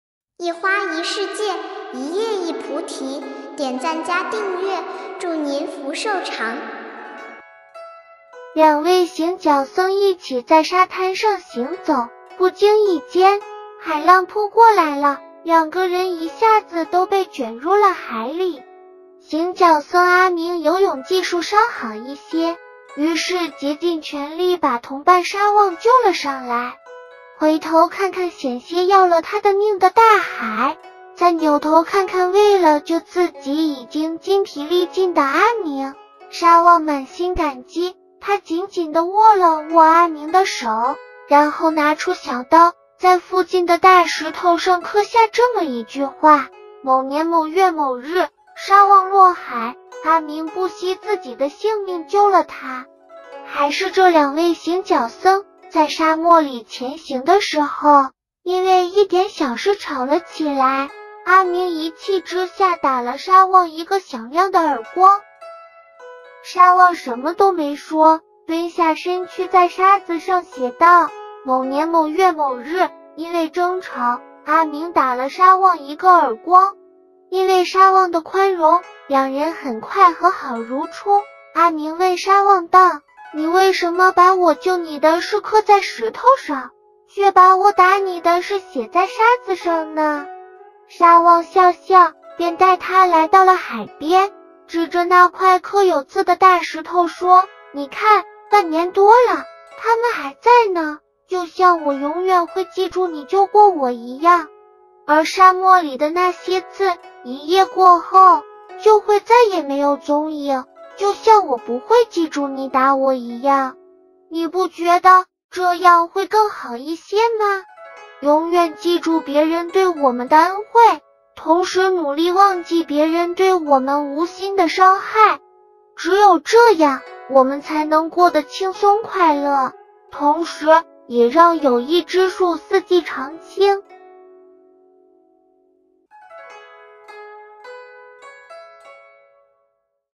小和尚讲故事：記住與忘卻#佛教
永遠記住別人對我們的恩惠，同時努力忘記別人對我們無心的傷害。只有這樣，我們才能過得輕松快樂，同時，也讓友誼之樹四季常青。